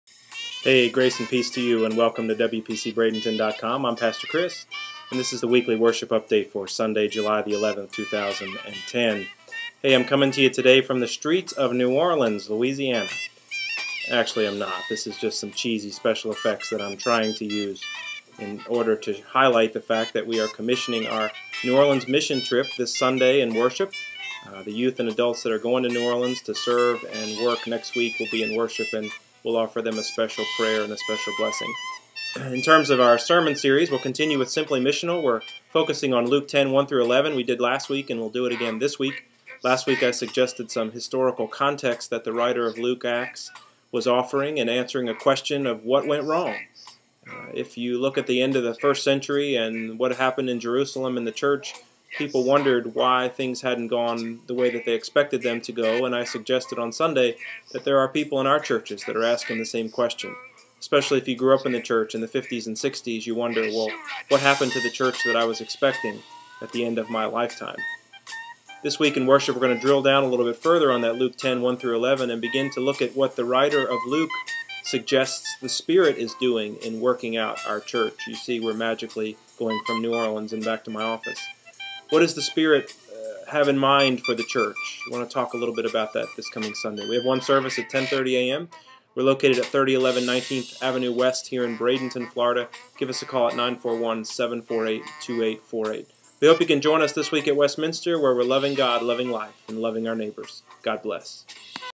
WPCV.07.11.10.mov
The weekly worship update for Sunday, July 11, 2010.